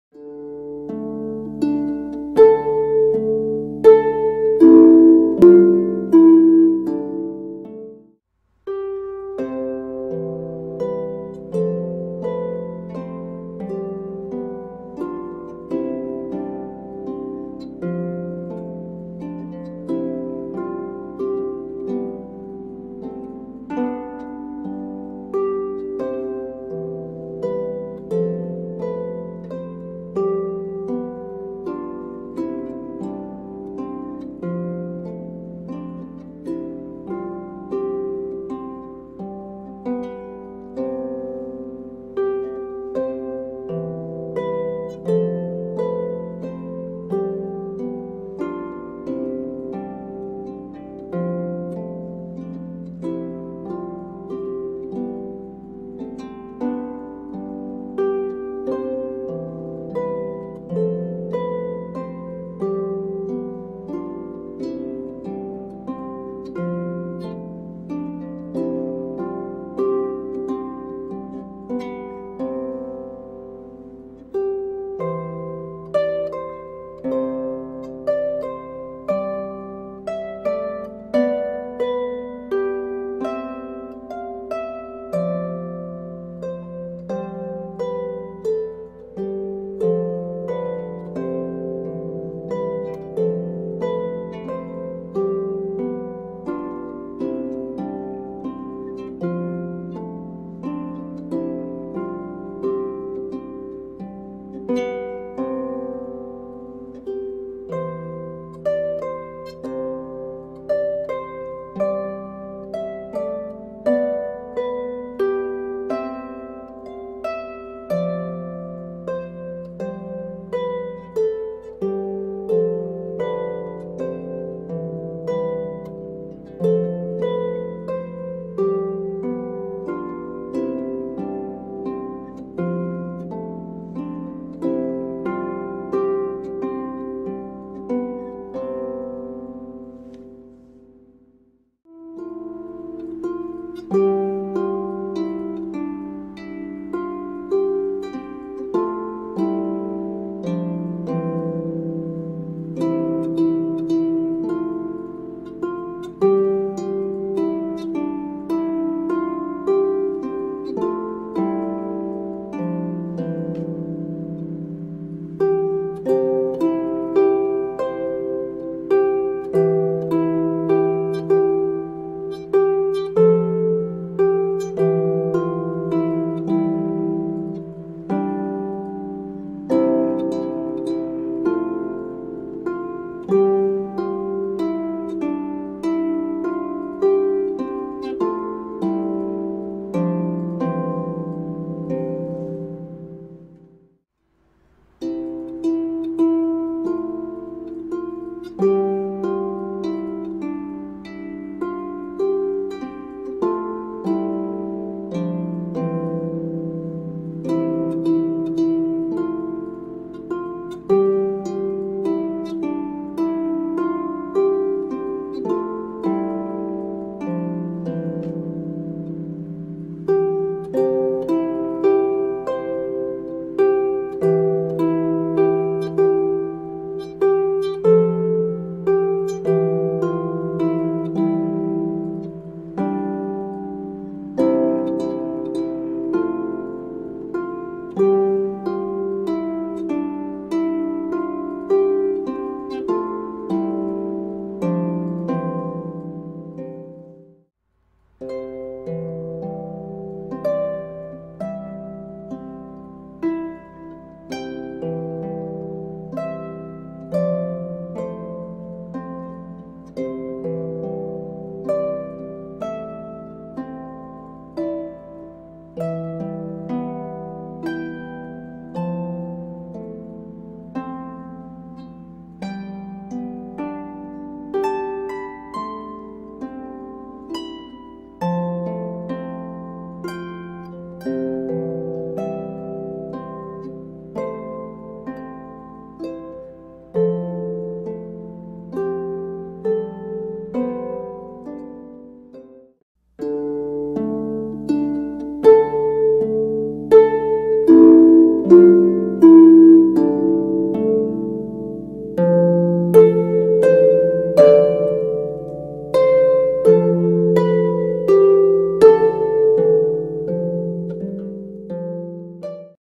HOLIDAY FAIRY MOUSE ON MUSHROOM - WATERCOLOR PAINTING TIMELAPSE
In this video I show my process for creating my original design of a Holiday themed fairy mouse on a cute red mushroom! I hope you enjoy watching how I create my artwork!

Supplies Used: Strathmore 140 lb cold press watercolor paper, Winsor & Newton watercolor paint pans, Dr. Ph Martins watercolor inks, Ahmarelle watercolor paint pans, Grumbacher watercolor round brushes, Micron black 1 mm pen.

All the music you hear in this video is played by ME on my Dusty Strings Ravenna 34 Harp! All are harp arrangements of classic and folk tunes. Check out the playlist below to view my harp playing videos!